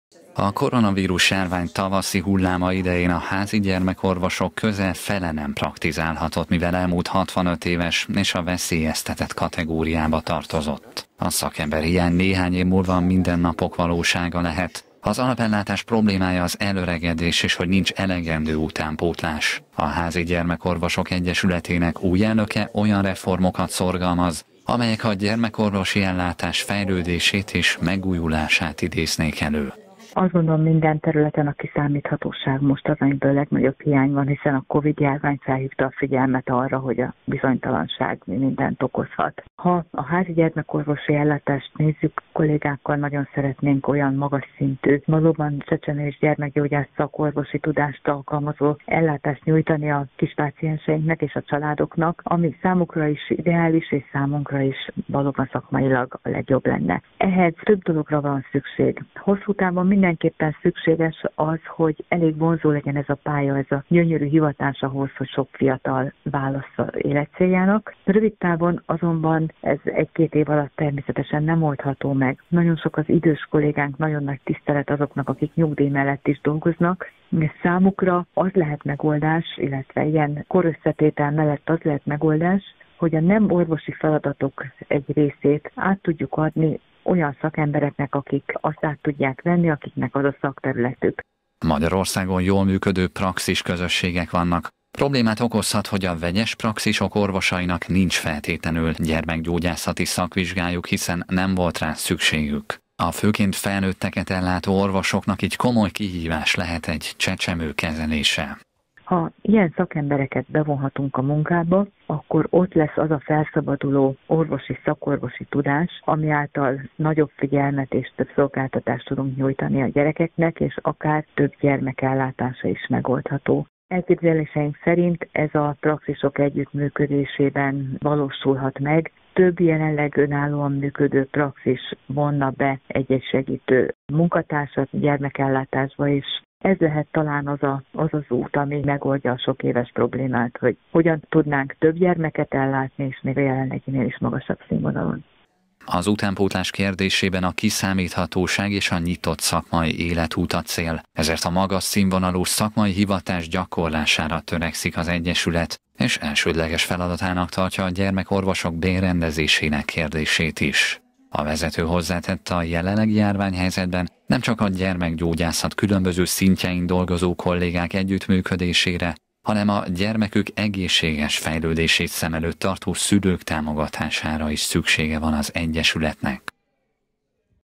Reformot szorgalmaznak - Kölcsey Tv
A gyermekorvosi alapellátás problémája az elöregedés és az, hogy nincs elegendő utánpótlás. Emiatt olyan reformok bevezetését szorgalmazza a Házi Gyermekorvosok Egyesületének újonnan megválasztott elnöke, amely kiszámíthatóságot és nyitott szakmai életutat jelenthet nekik.